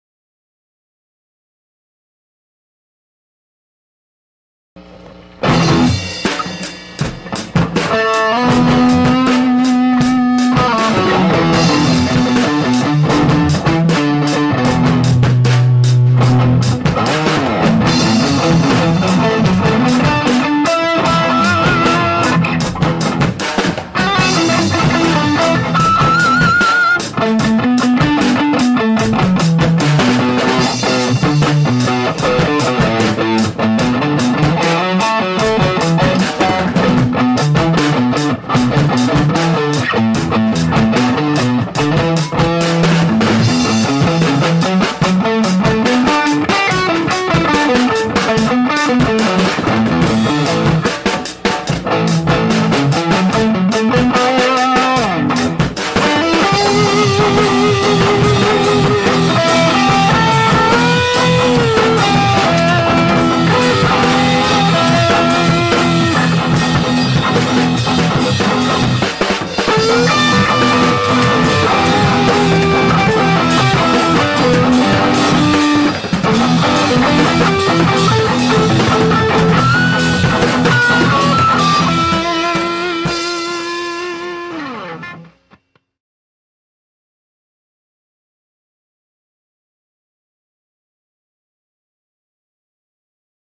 Jeff McCurley - Instrumental Rock Jam
Jeff McCurley from Tulsa, Oklahoma doing a random, instrumental, heavy rock jam.